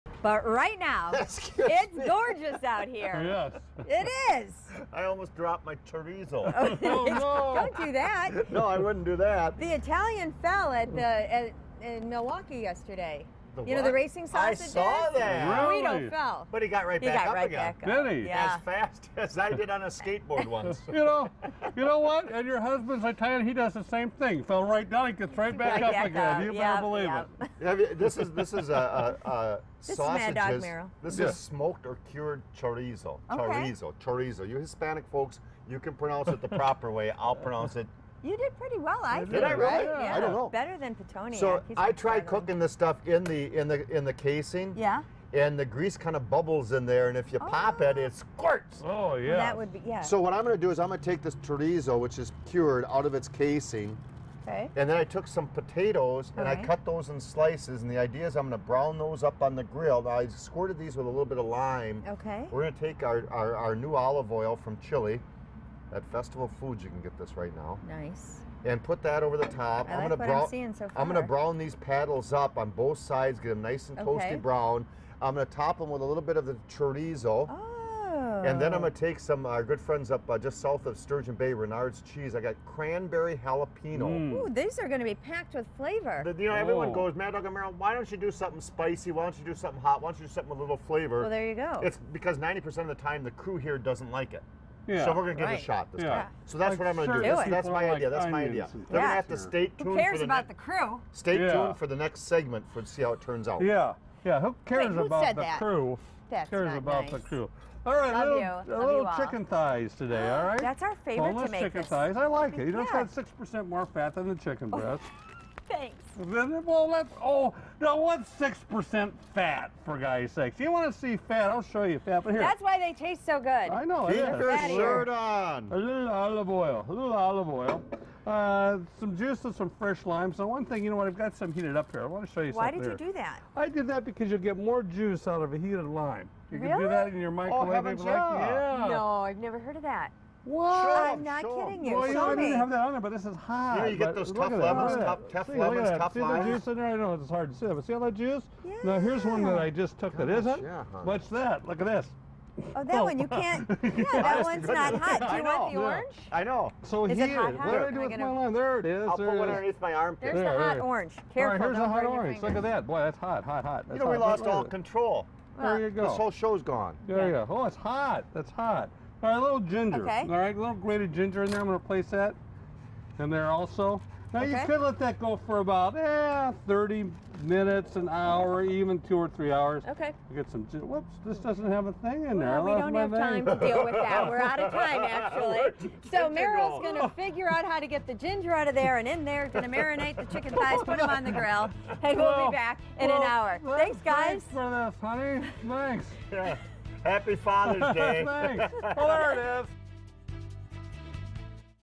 GDW7_mad_dog_and_merrill_sausage_potatoes_and_chicken
Mad Dog and Merrill are on the weather deck this morning grilling up sausage, potatoes and chicken thighs.